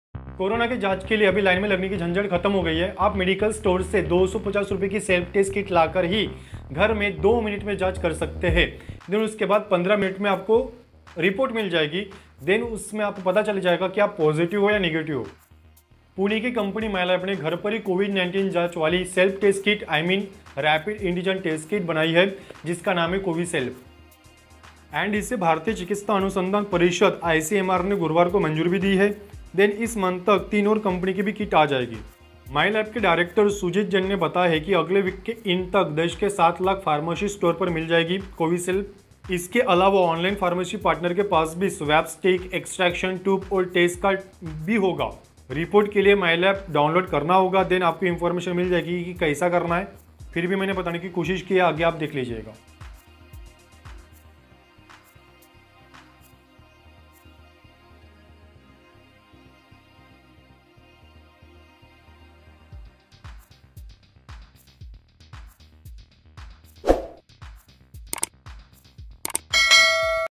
ICMR approved Covid-19 Test at Home With Rapid Antigen ( CoviSelf ) In Hindi. | Mylab | India
Pune lab develops India’s first home Covid test kit; MyLab CoviSelf at chemists’ for Rs 250

 The CoviSelf TM COVID-19 Antigen test is a self-test kit for detection of protein antigen from SARS-CoV-2 in direct nasal swab samples from individuals with or without symptoms of  COVID-19.

 The Indian Council of Medical Research (ICMR) has given its approval to the country’s first home-based, self-use rapid test for Covid-19, the creators of the test, MyLab Discovery Solutions, announced on Thursday.

The self-use test kit can be used by symptomatic individuals and immediate contacts of confirmed cases as per the ICMR’s guidelines.

The test kit, named CoviSelf, will cost Rs 250, and can be bought without a prescription from pharmacies and online.